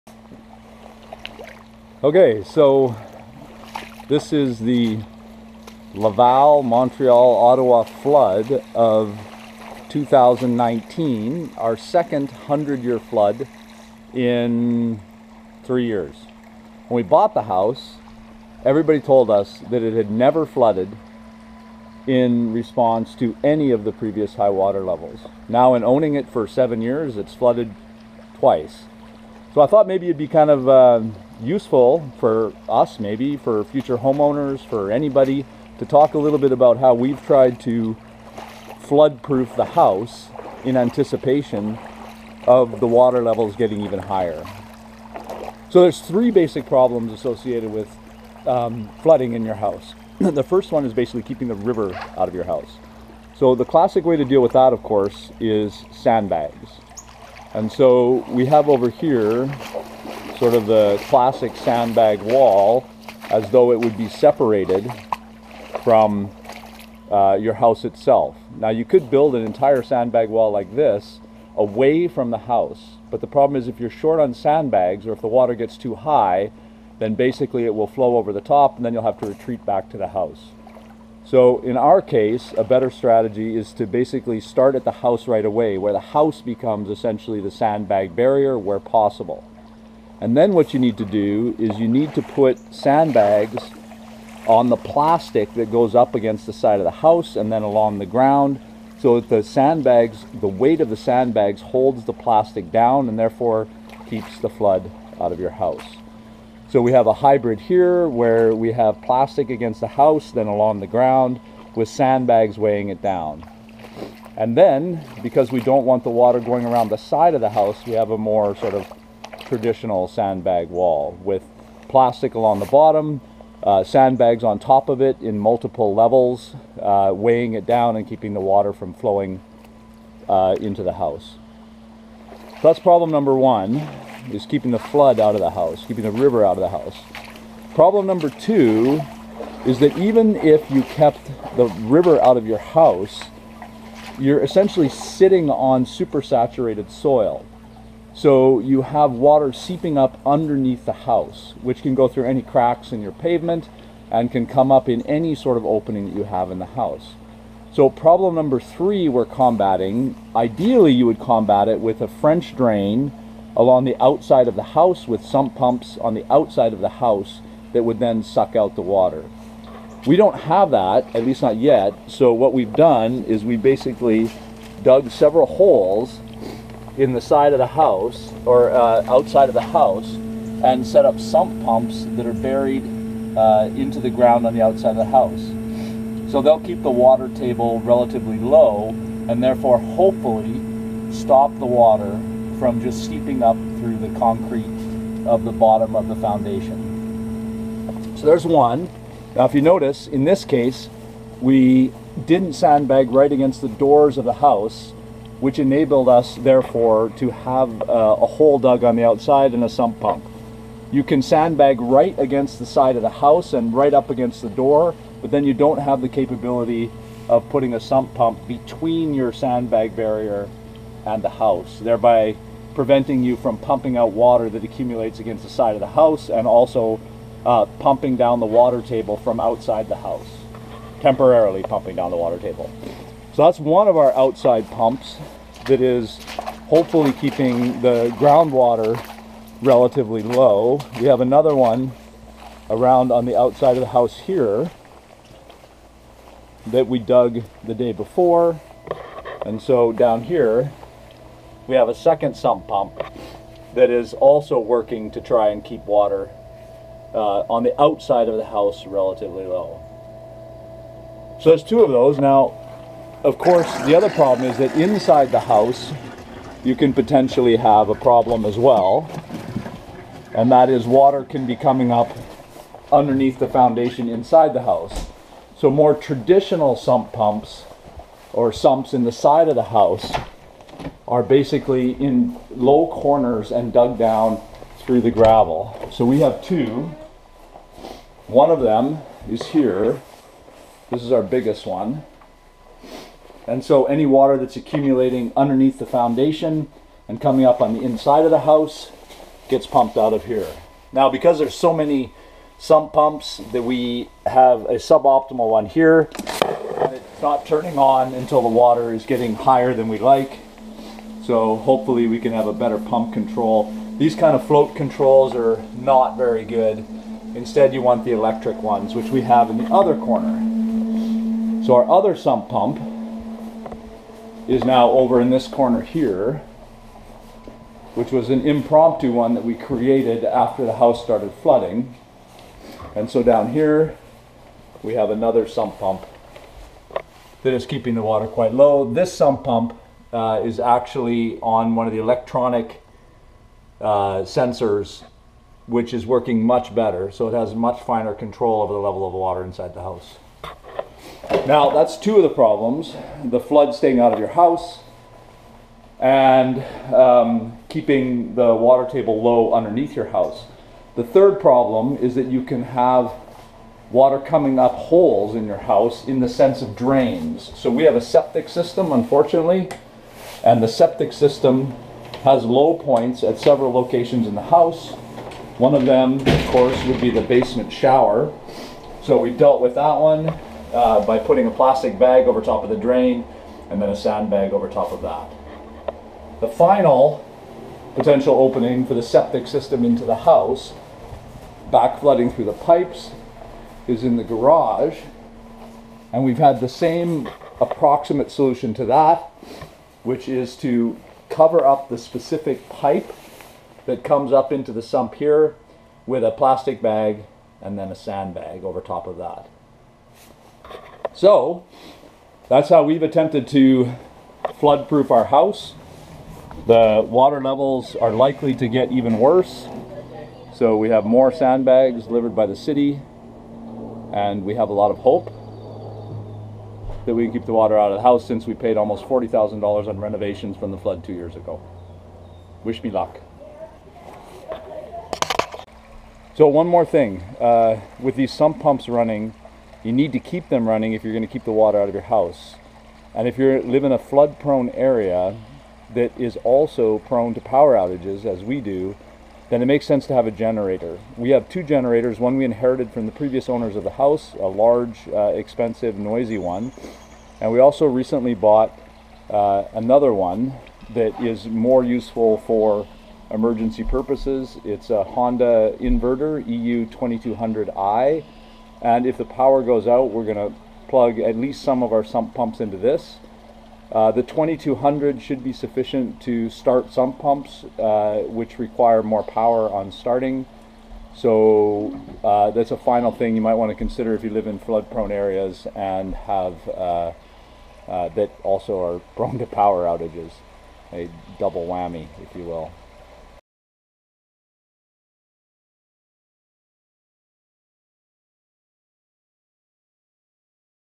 How to Flood-Proof Your Home I
For a post-flood summary of everything - look here - it has more info in a more concise way

This was a first one-take video I just made about trying to flood-proof our home for the 2019 Laval-Montreal-Ottawa flood, and the THREE big problems you need to solve when doing so.